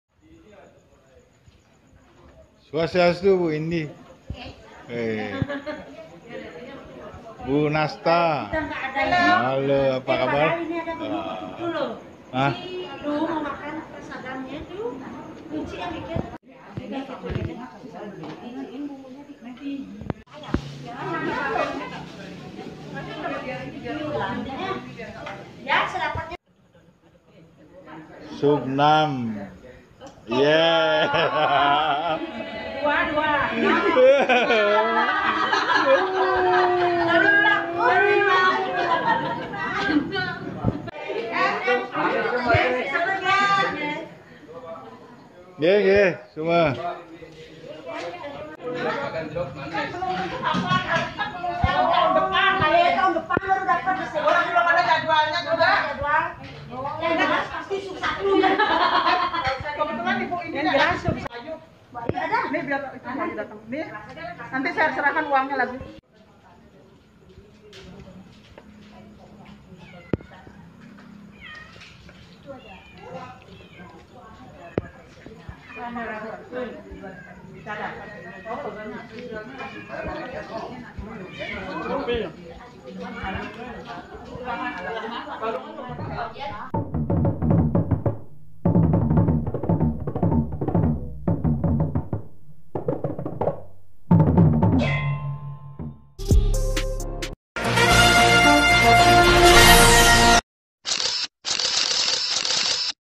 Selesai Tugas Rahine Purnama Sasih Kelime, Minikmati Prasadam Ayam Betutu, Ketupat, Sayur
Tugas Tempek SKS, Sub 6.
Selasa, 08/11/2022.